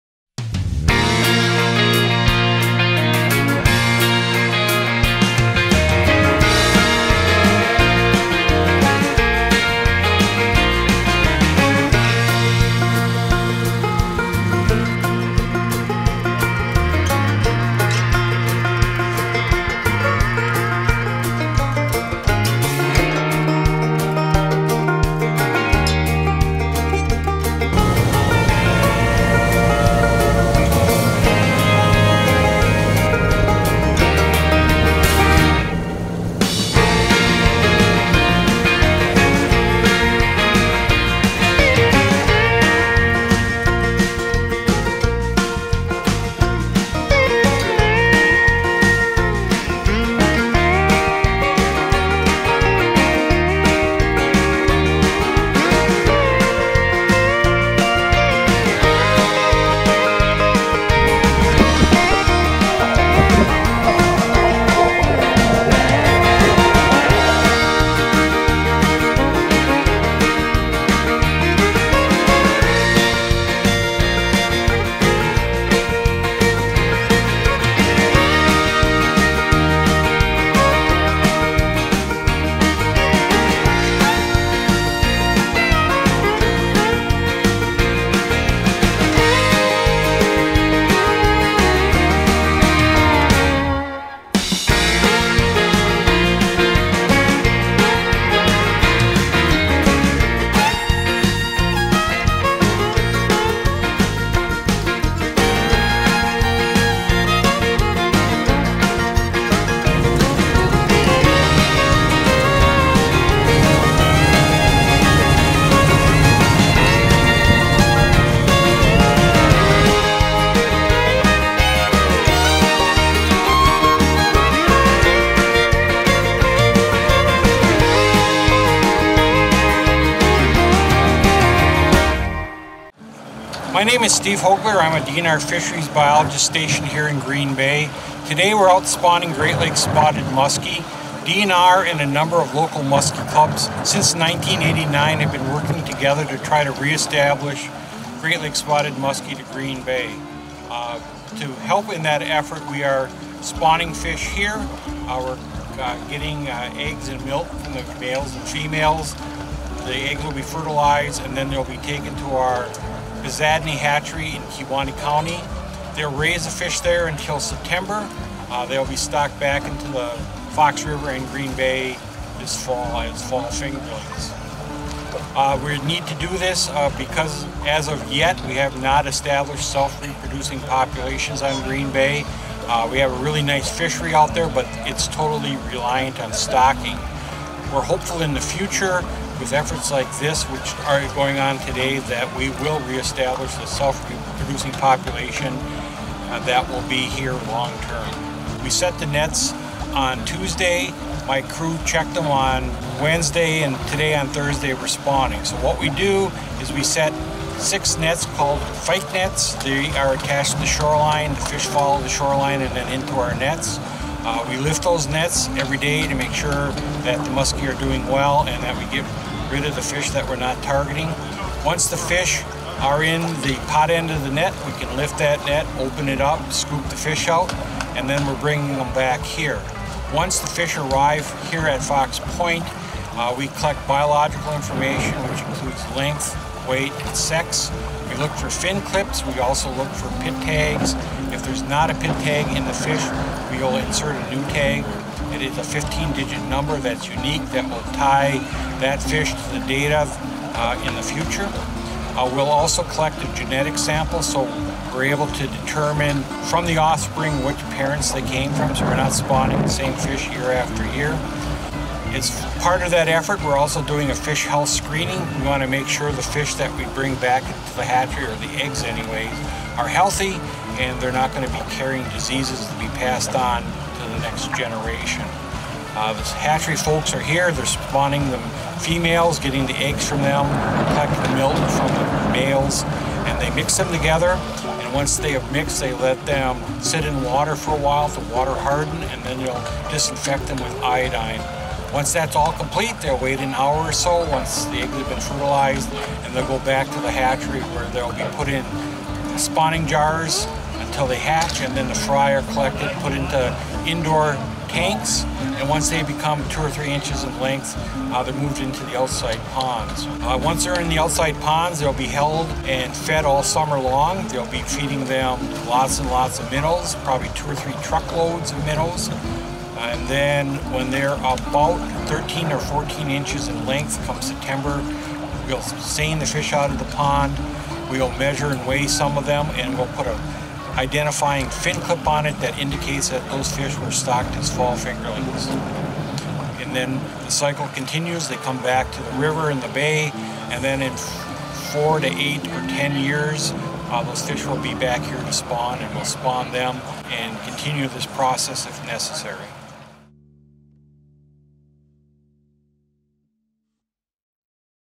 Muskie Spawning with the Wisconsin Department of Natural Resources
On Thursday May 16th we sent our TRS Media guy Zach with the Wisconsin DNR to film the process of collecting, researching, and spawning the Spotted Muskellunge in the bay of Green Bay!

Great big thanks to the DNR for everything they do for nature and the fish we all love to try and catch!